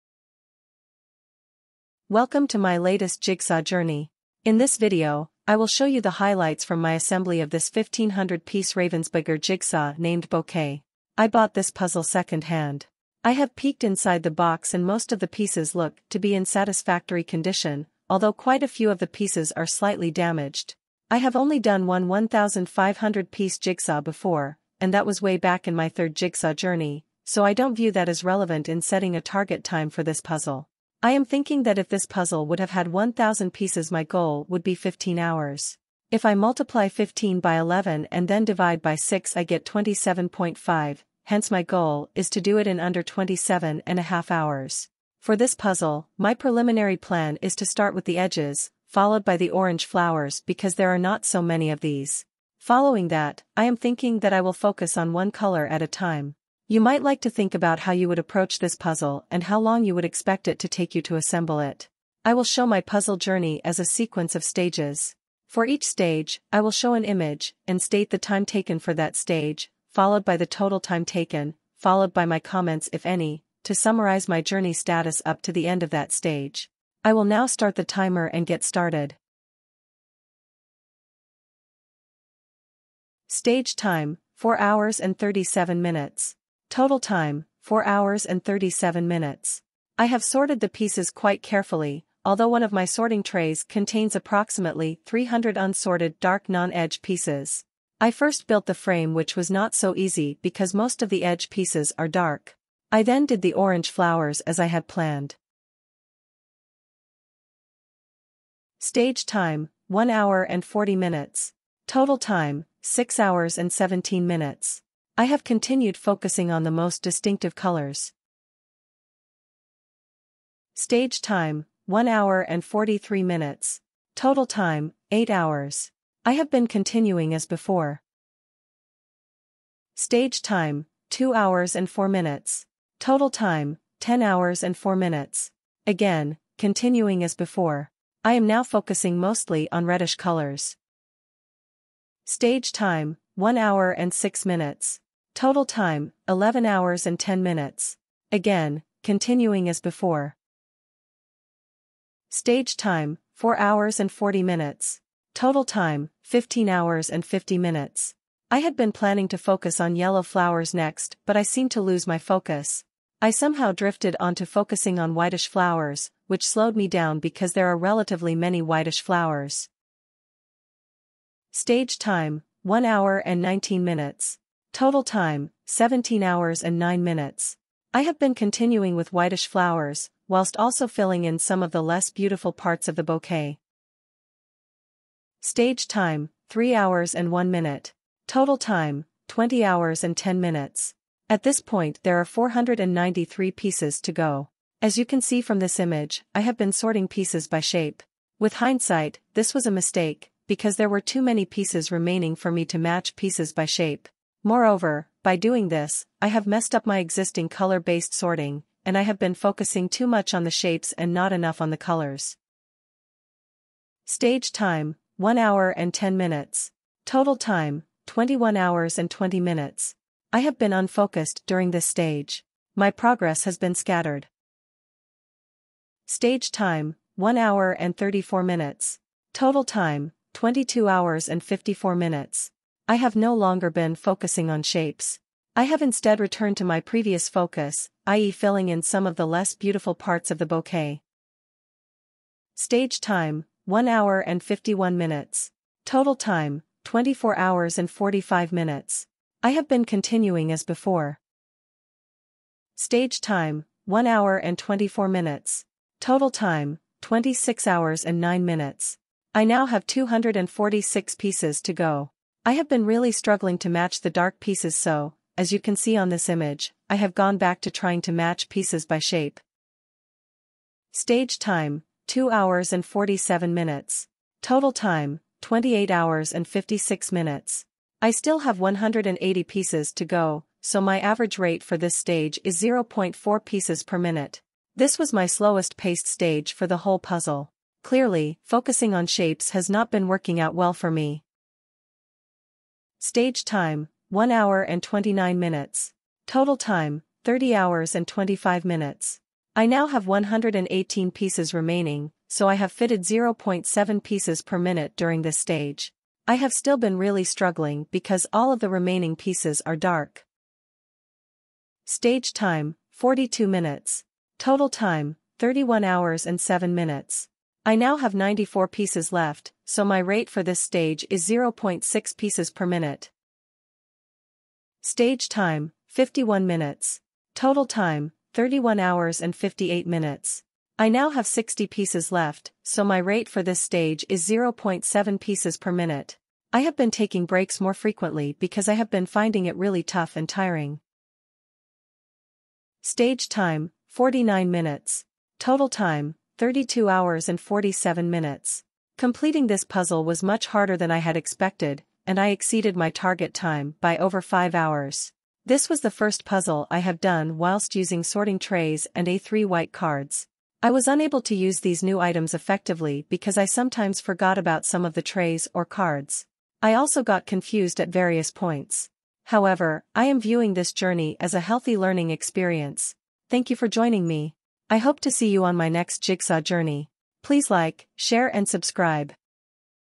Bouquet 1500 -- Jigsaw Journey 49
Please join me on this jigsaw journey, showcasing my assembly of this 1500-piece Ravensburger jigsaw puzzle, named "Bouquet".